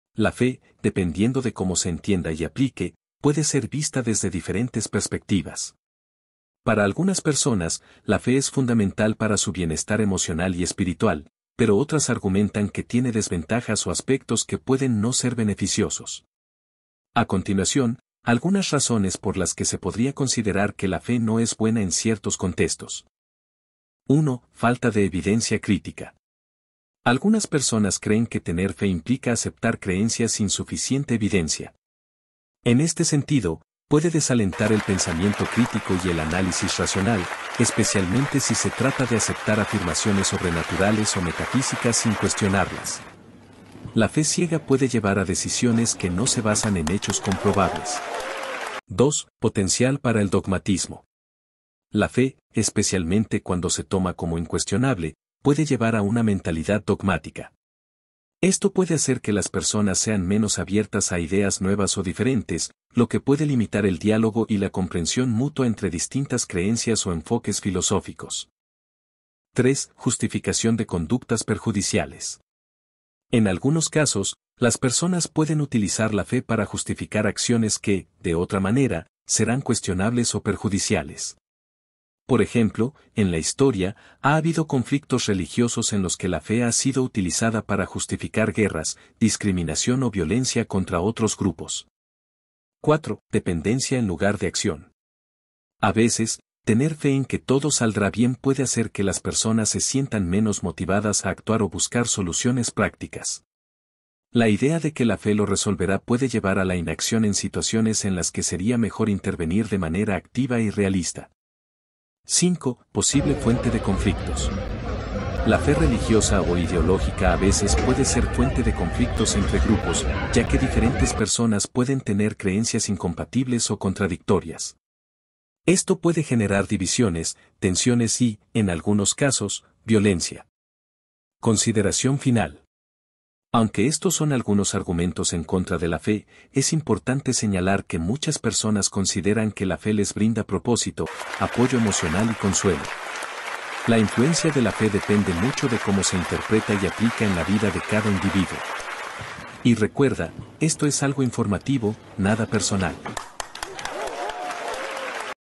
El Lado NEGATIVO de la FE 😱
AL ESTADO LE CONVIENE QUE EL POBRE TENGA FE EN DIOS U OTRA COSA, ASI DE ESA MANERA EL POBRE NO SE QUEJA DE SU SITUACIÓN YA QUE LE CONTARON SOBRE LA VIDA ETERNA DESPUÉS DE LA MUERTE.